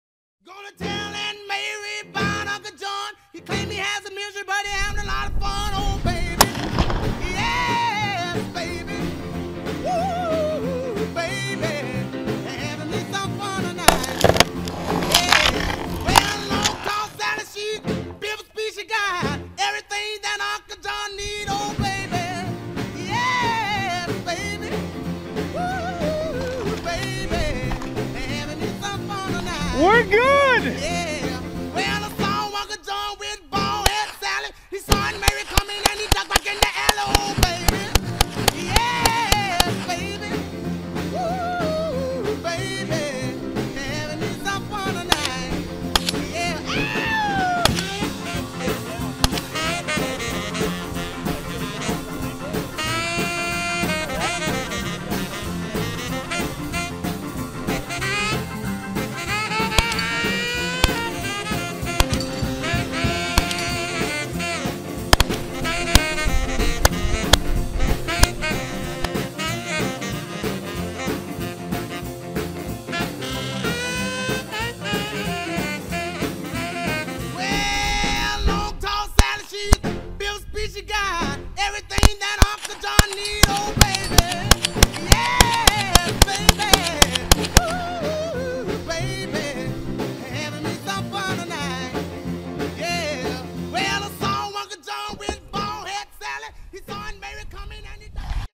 50 BMG and a AR-22 GoPro Edit
Shooting rocks and poker chips